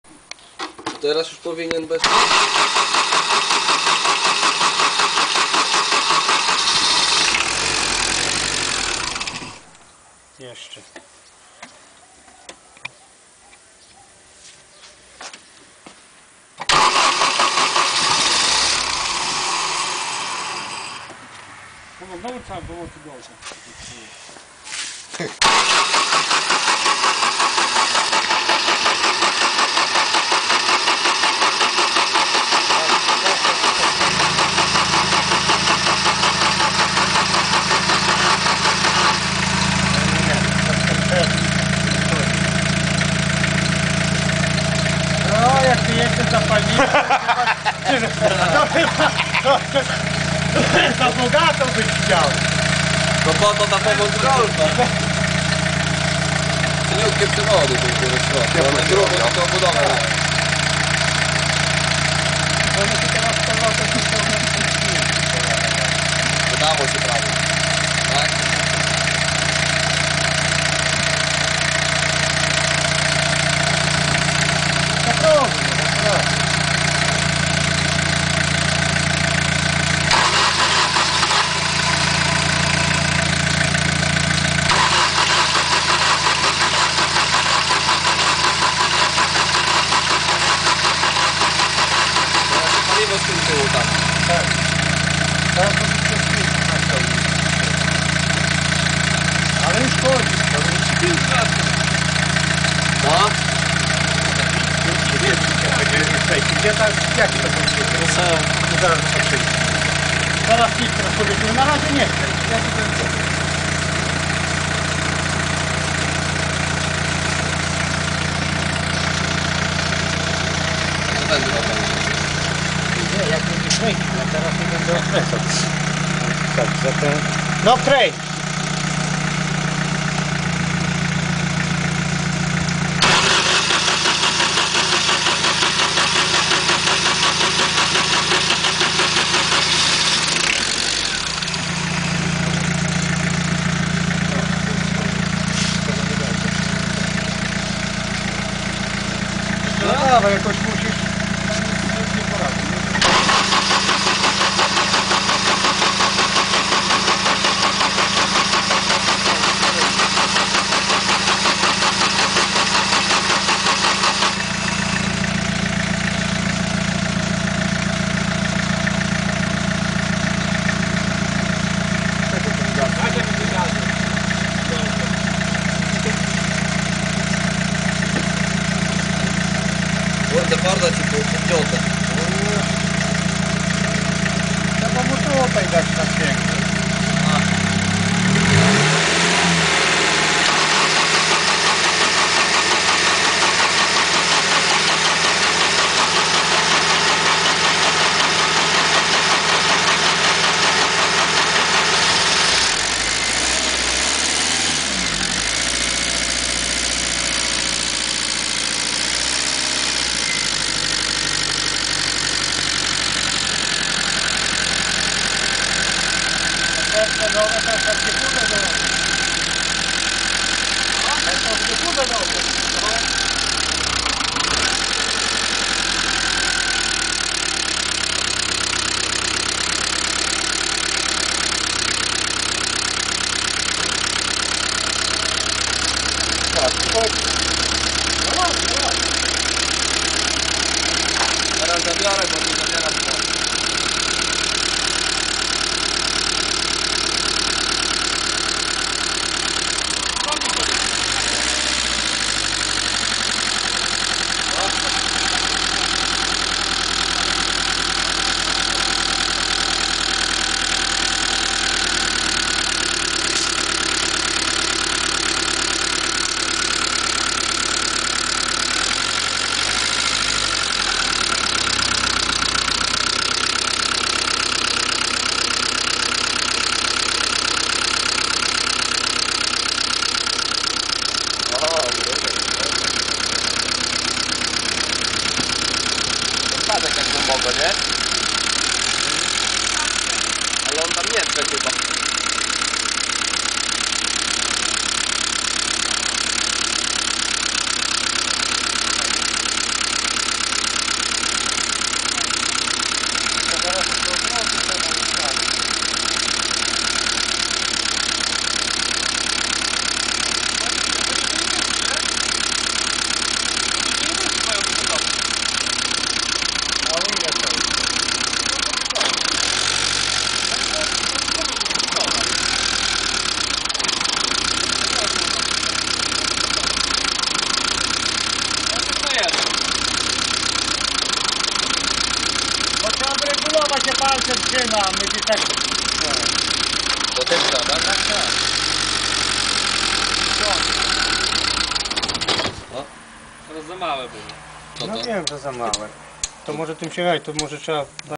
"FORD SIERRA 1.8 TD - REAKTYWACJA" - cz. 10
FORD już odłączony od ropy z butelki. Podłączony do swojej ropy odpalił z pomocą GOLFa i pojechał sobie w siną dal :D